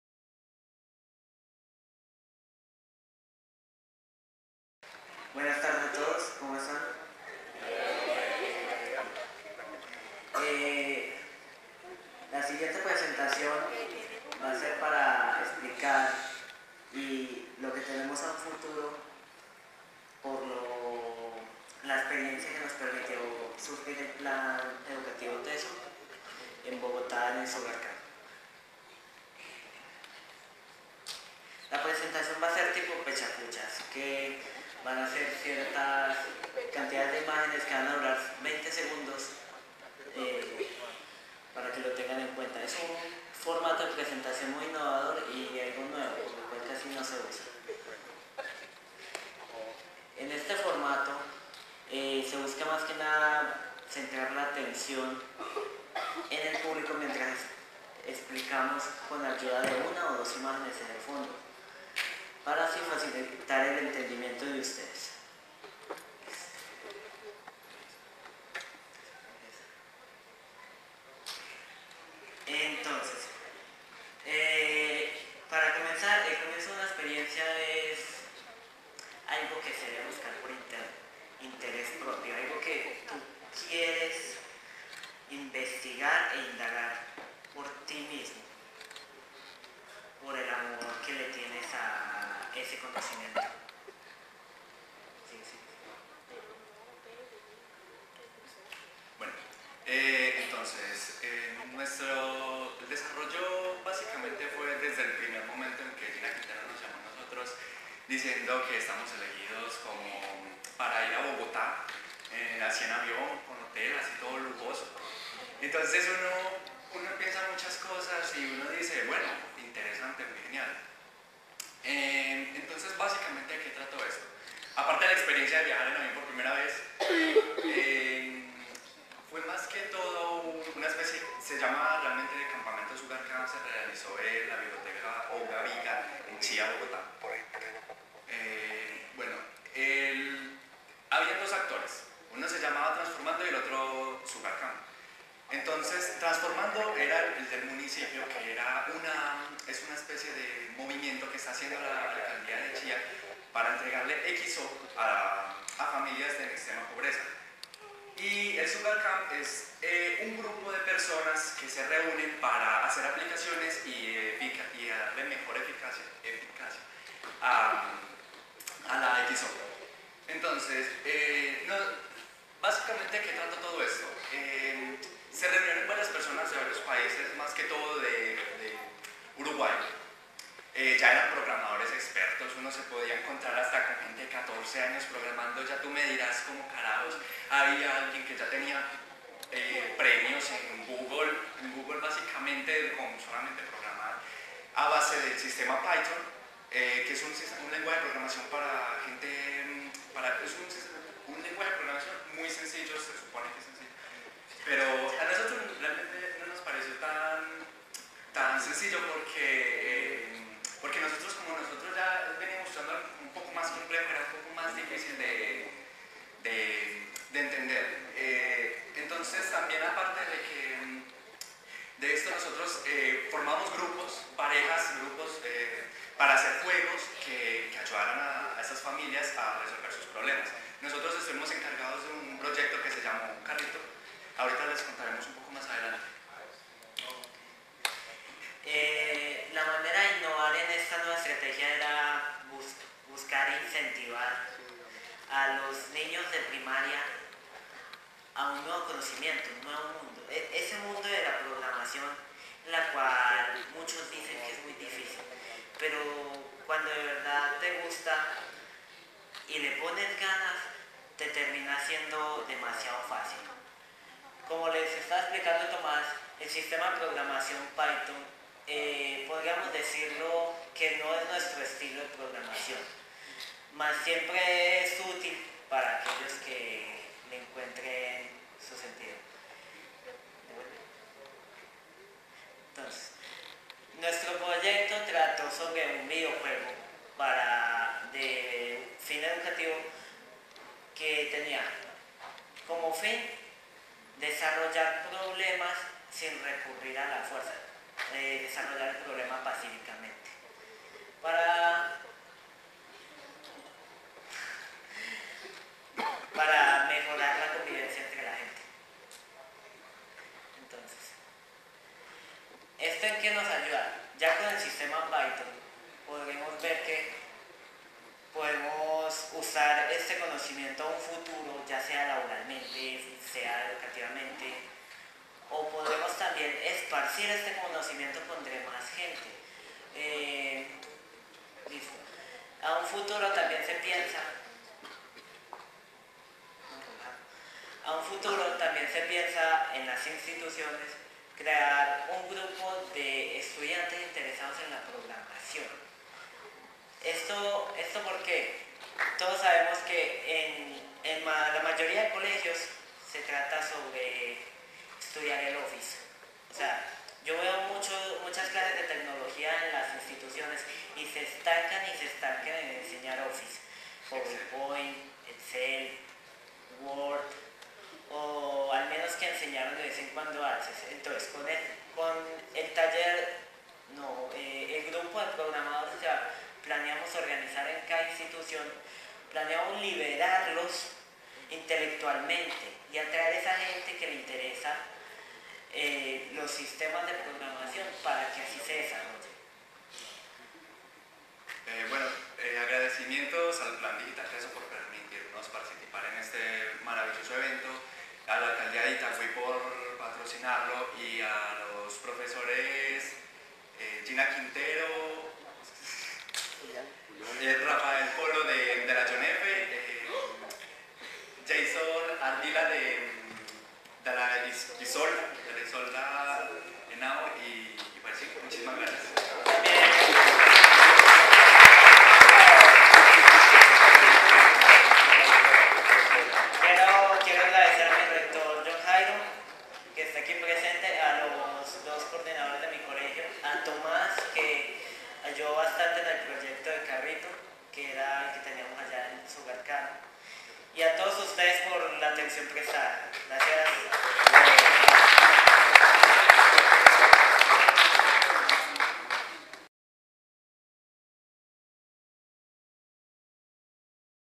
Innovando con TIC, Andres Quiróz y Tomas Arango, Monitores TESO
Nuestros monitores TESO, de las I.E. Orestes Sindicce y la I.E.
Simón Bolívar, exponen en el encuentro TESO